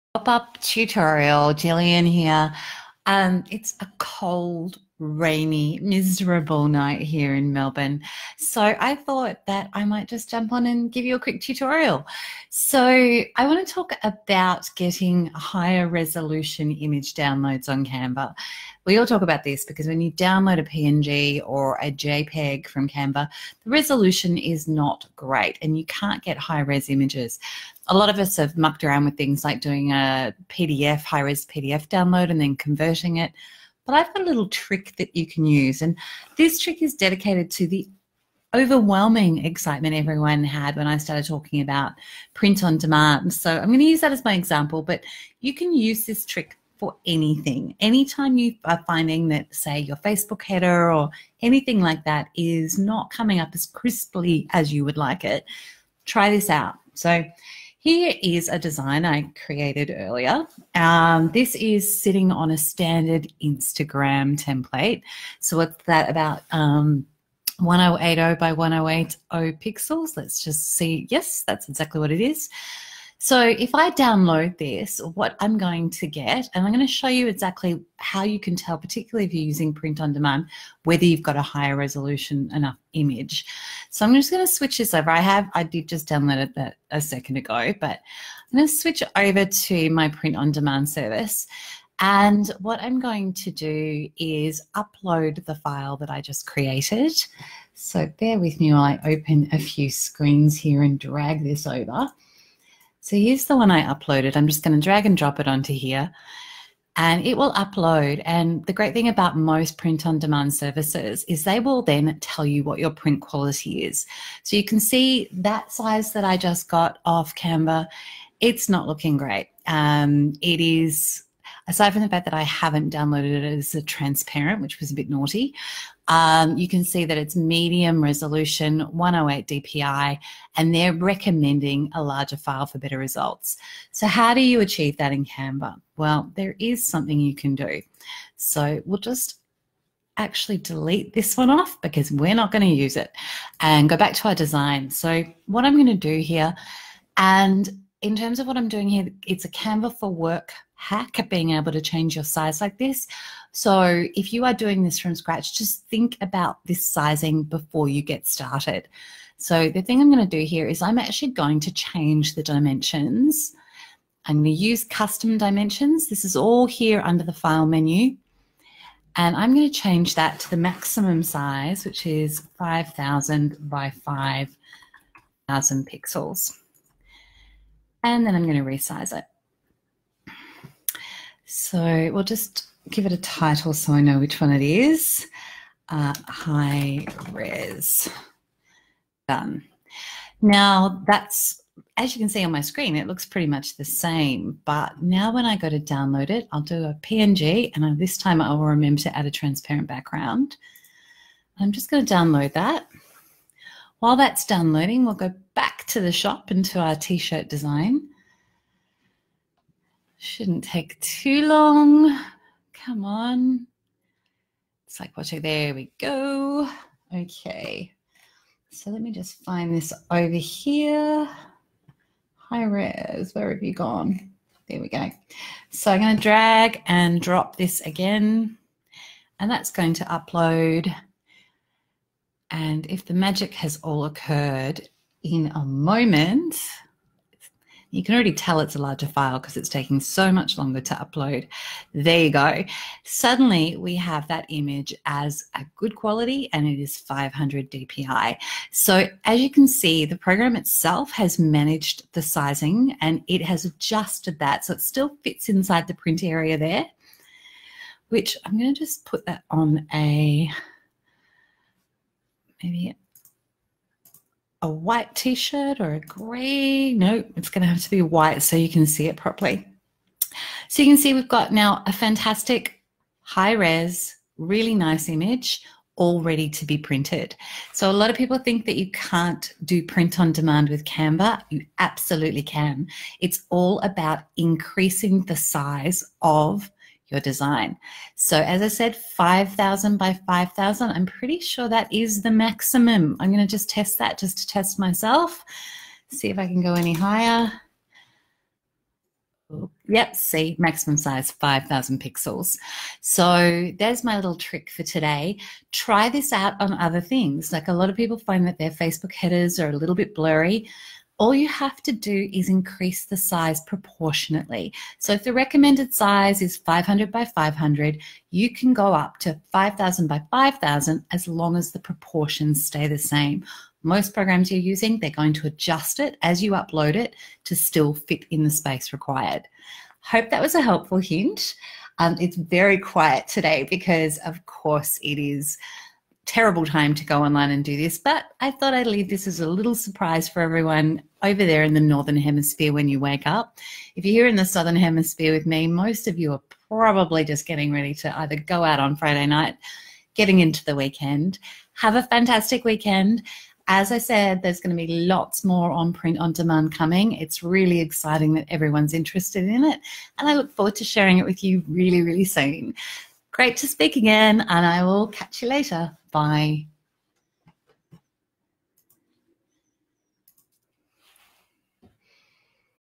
How to download higher resolution images with Canva
One of the challenges I hear about with Canva is getting higher resolution images. Whether you need this to improve the quality of a Facebook Header or if you are creating POD Print on Demand T-shirts and other merchandise, this quick tutorial recorded live for my Facebook group will help.